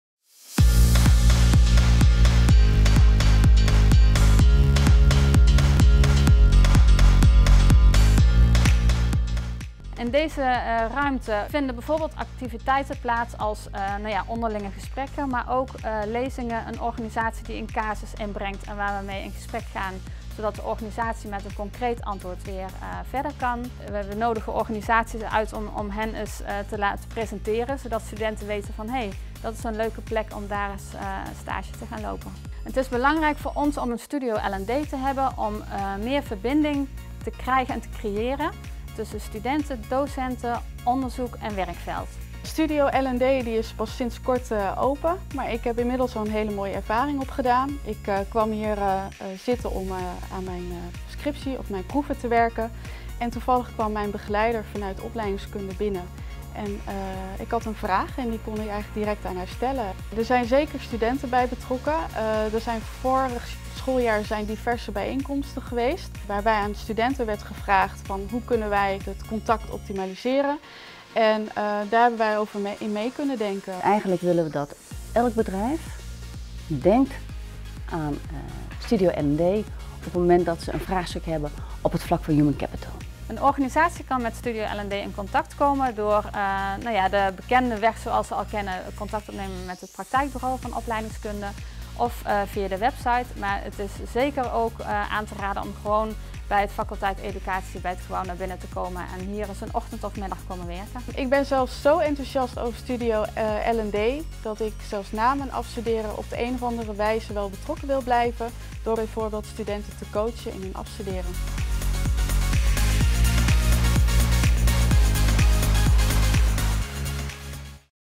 Wat is Studio L&D?
Studio L&D; dé plaats waar studenten, docenten, alumni en werkveld (L&D, Opleidingskunde, HRD) samen komen om van en met elkaar te leren.